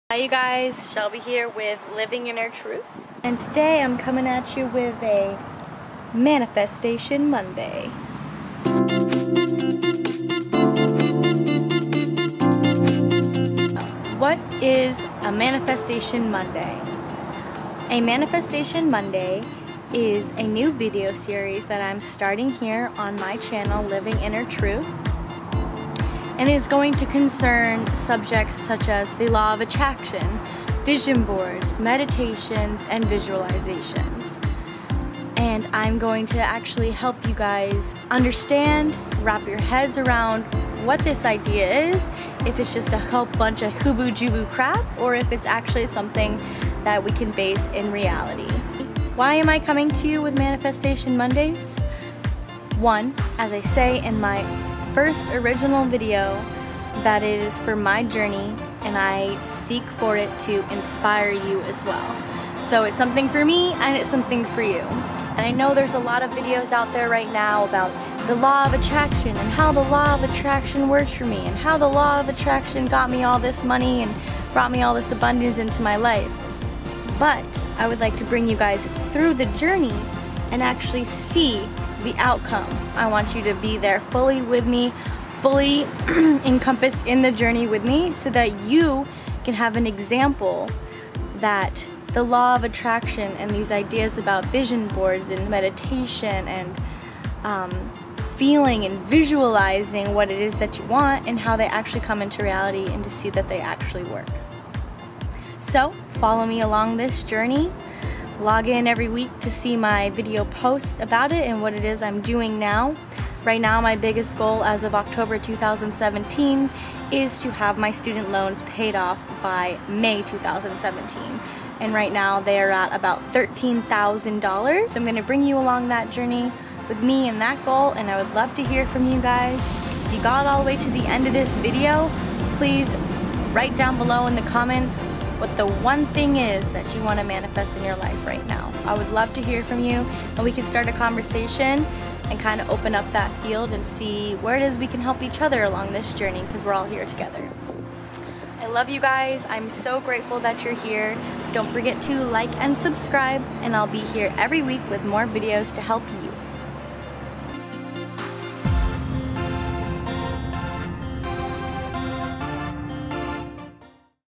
Journey with the Law of Attraction
Hello World! Welcome to my newest series called Manifestation Mondays where I will discuss all matters concerning the law of attraction, meditation, visualization, and vision boards.

p.s if you got all the way down here. comment "I wish to manifest ________"